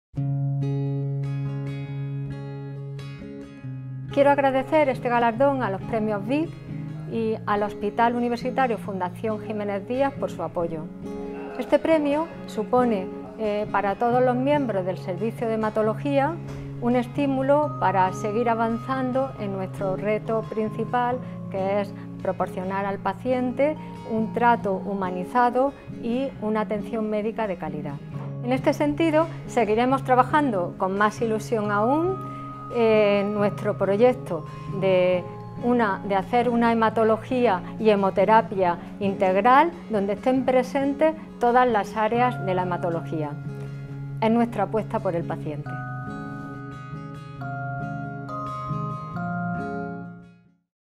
La Dra. Pilar Llamas agradece el Premio Best in Class al servicio de Hematología de la FJD
El Hospital Universitario Fundación Jiménez Díaz ha recibido tres  Premios Best in Class -otorgados por Gaceta Médica y la Universidad Rey Juan Carlos-, a los mejores servicios en Urología, Hematología y Nutrición. La Dra. Pilar Llamas, jefe de servicio de Hematología, valora así el premio recibido.